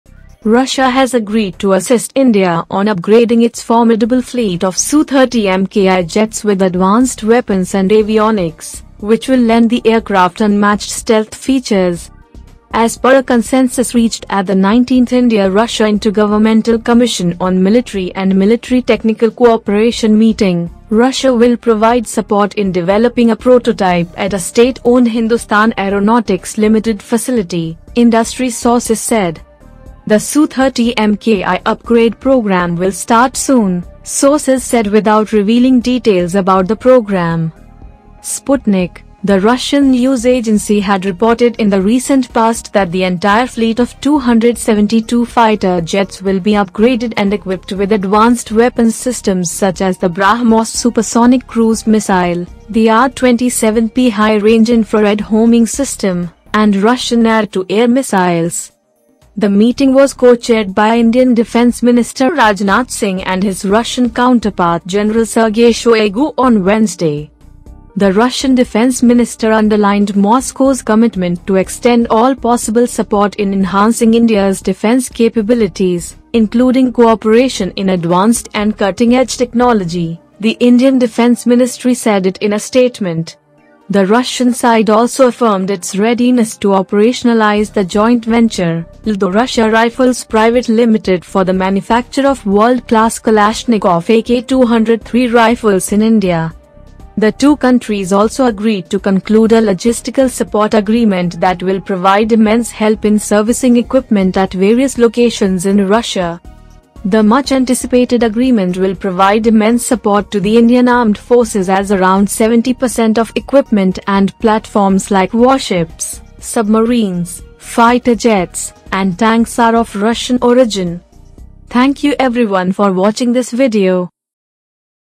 Super Sukhoi Prototype is Coming Very soon with the Assistance of Russia
Russia has agreed to assist India on upgrading its formidable fleet of Su-30 MKI jets with advanced avionics and other systems, which will lend the aircraft unmatched stealth features.
As per a consensus reached at the 19th India-Russia Inter-Governmental Commission on Military and Military Technical Cooperation meeting, Russia will provide support in developing a prototype at a state owned Hindustan Aeronautics Limited facility, industry sources said.
“The Su-30 MKI upgrade programme will start soon", sources said without revealing details about the programme.
This up gradation programme is named as Super Sukhoi.